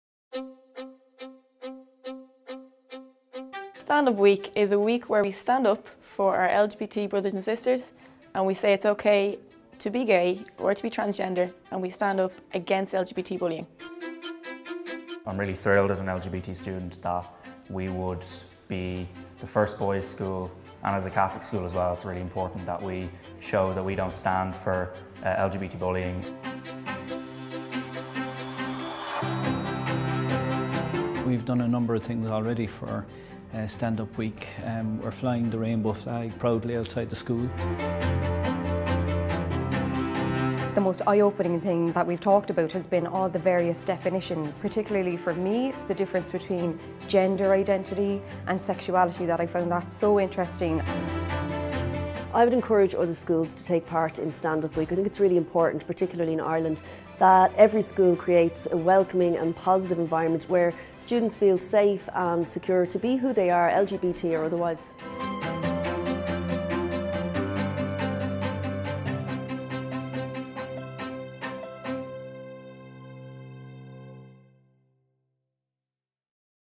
Stand Up Awareness Week 2017
Stand Up! Week tackles homophobic and transphobic bullying by increasing awareness, friendship and support for LGBT students by other students. for more details.